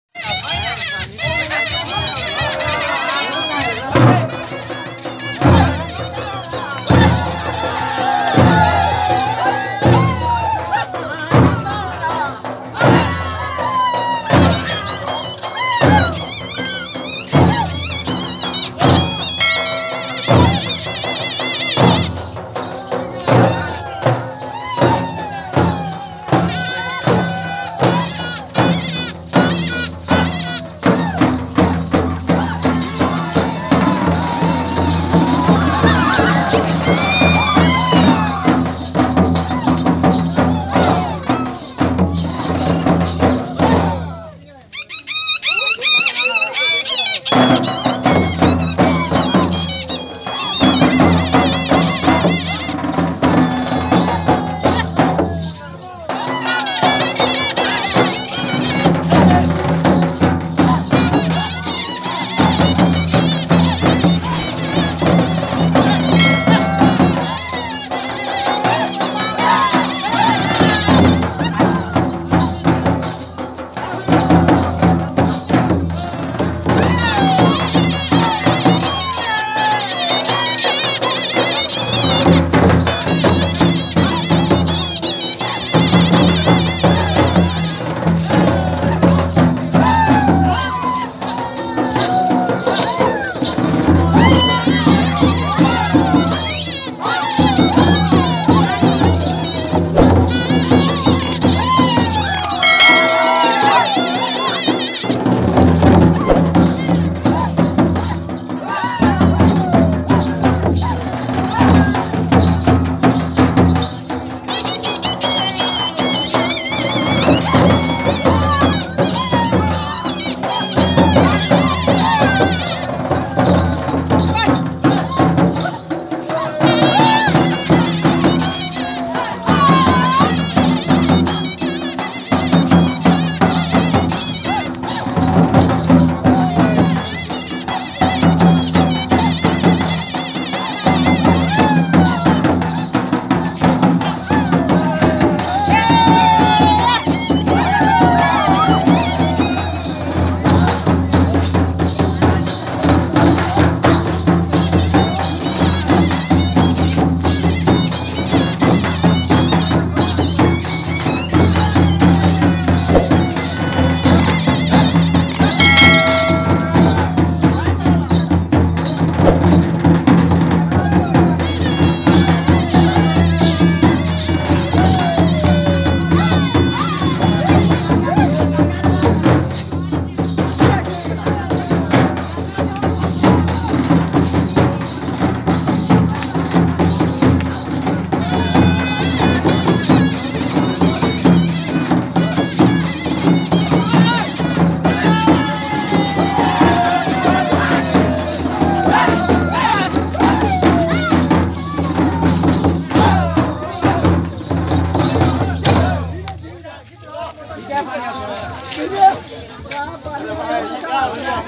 Jhumair kar jhankar group Parghani dance
🙏johar dosto .raur man kar. bich me.
Ek Parghani dance. dekhayk jathi.
dhaniya bad 🙏